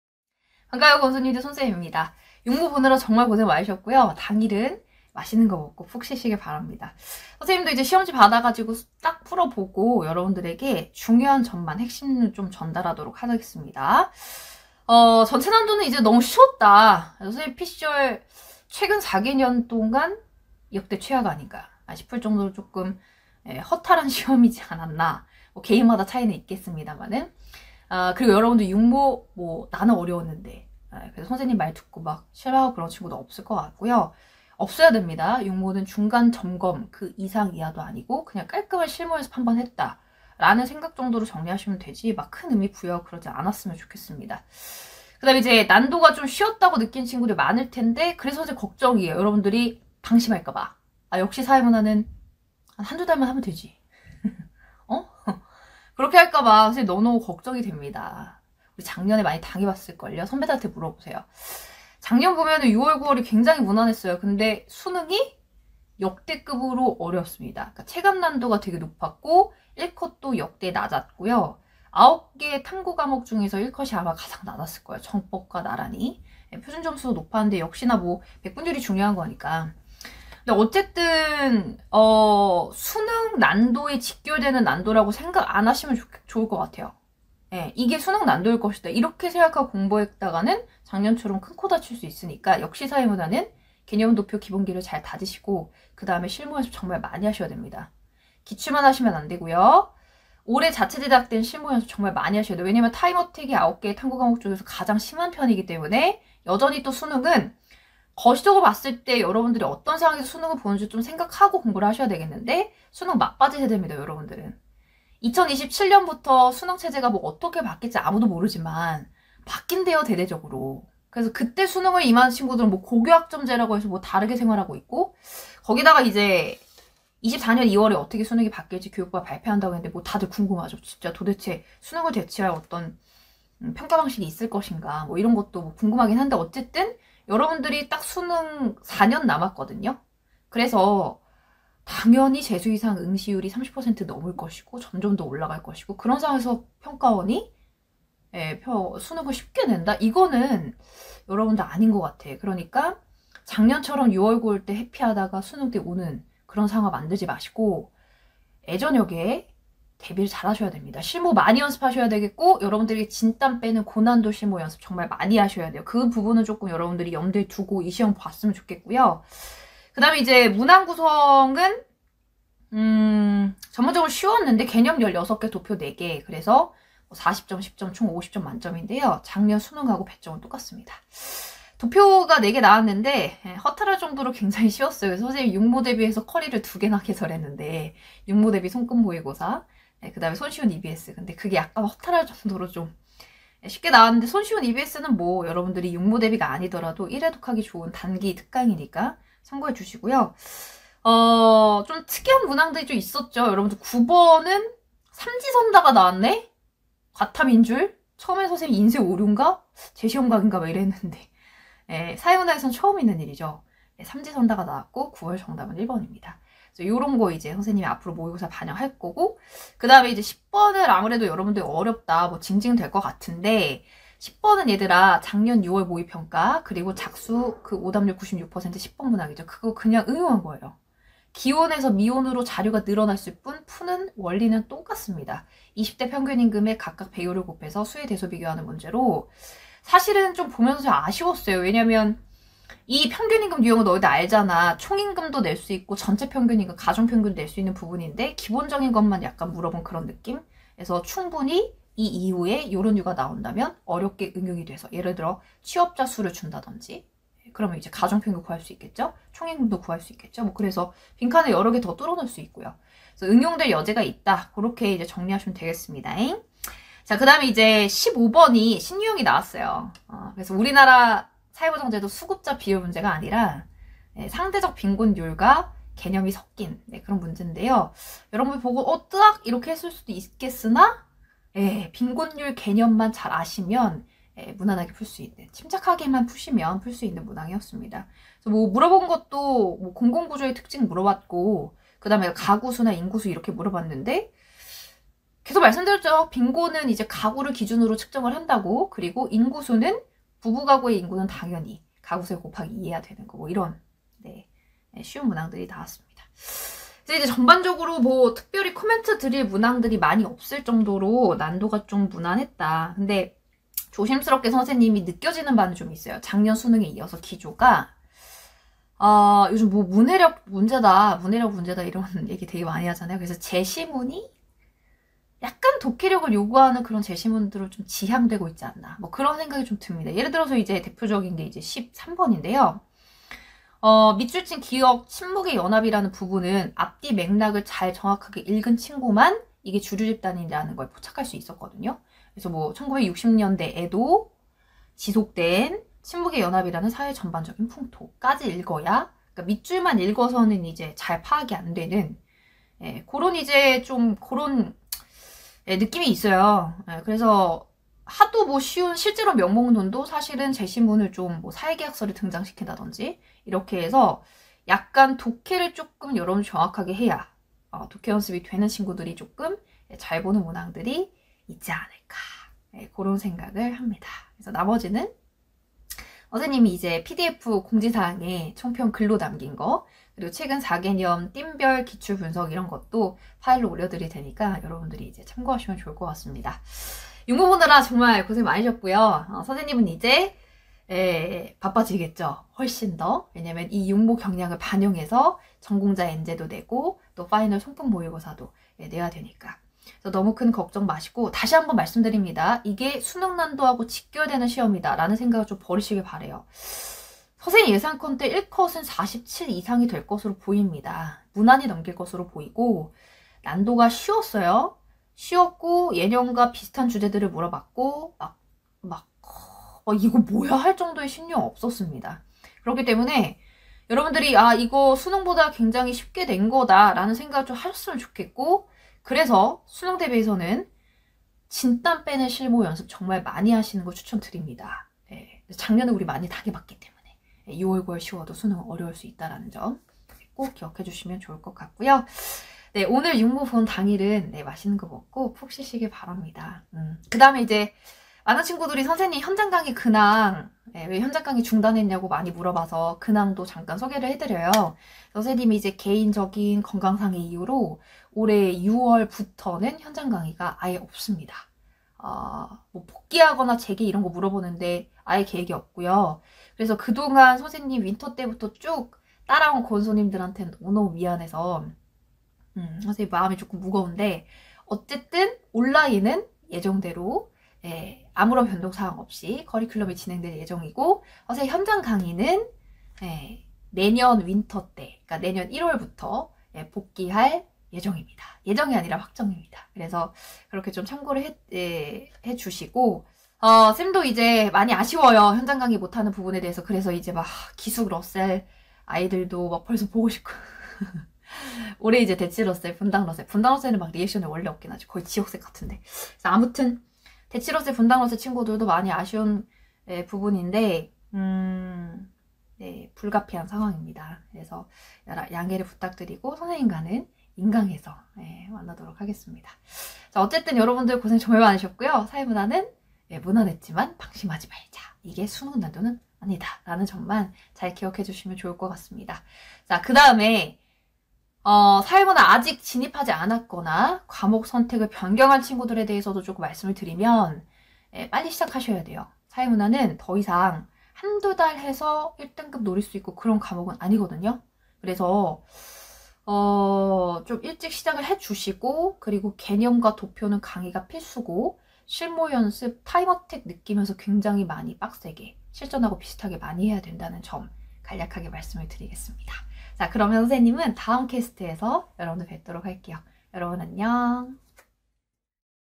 [메가스터디] 사회문화 손고운쌤 - 고운샘의 사회문화 6모 총평 (feat. 현강 중단 사유)
고운쌤의 6월 모평 사회문화 총평

# 6월 모평 총평
1. 난이도 : 매우 쉬운 편
2. 주목할 문항 : 9번(3지선다), 10번, 15번

그 밖에 출제 경향과 학습법까지!

- 사회문화 손고운 선생님을 더 알고 싶다면?